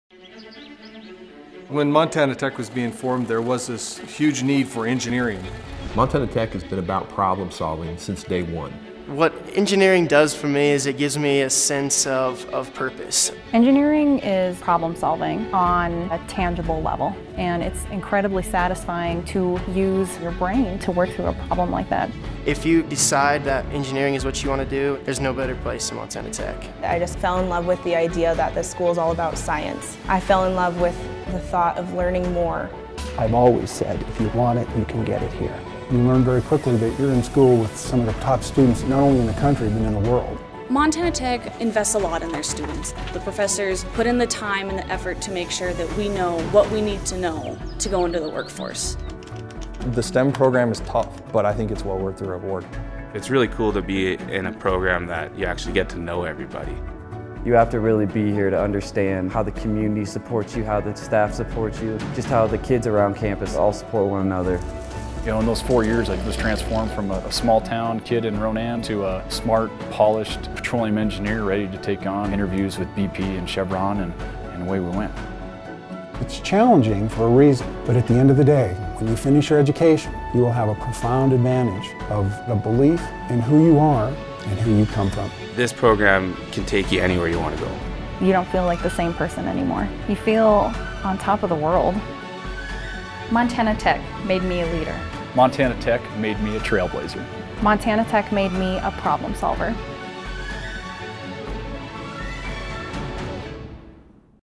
Explore Montana Tech's School of Mines & Engineering
Montana Tech School of Mines & Engineering offers degrees in Civil, Electrical, Environmental, Geological, Geophysical, Mechanical, Metallurgical & Materials, Mining, and Petroleum Engineering along with a degree option in Safety, Health & Industrial Hygiene.

As a graduate of Montana Tech's School of Mines and Engineering, you'll be a leader among your peers, respected for the quality of work you do each day, the skills you've mastered, and the deep foundation of knowledge you've gained through education and hands-on experience.

Montana Tech is the solution. Find your solution today.